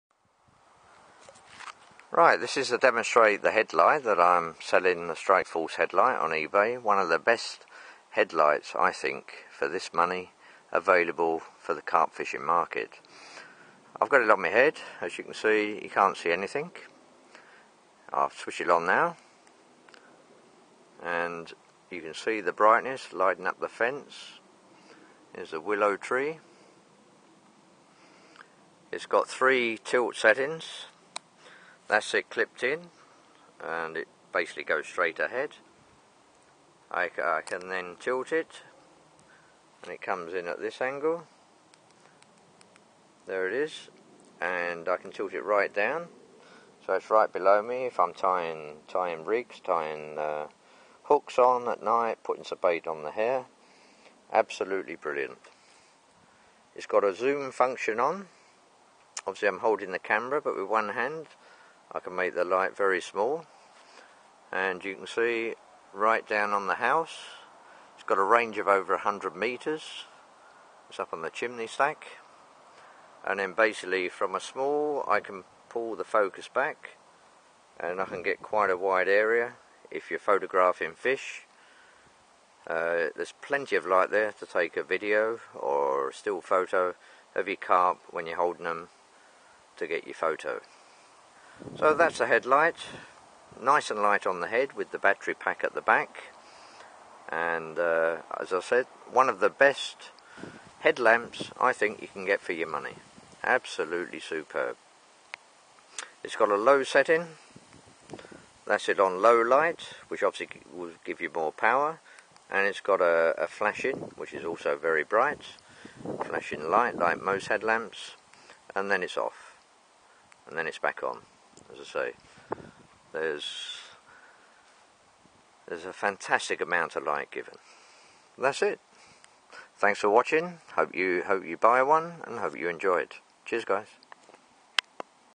Strikeforce head light demo .MP4
One of the brightest night carp fishing head lights available on the market. On sale now, just search for Strikeforce-carp-tackle on Ebay.